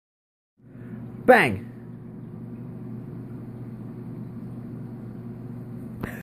Zazu plays dead
Zazu the caique playing dead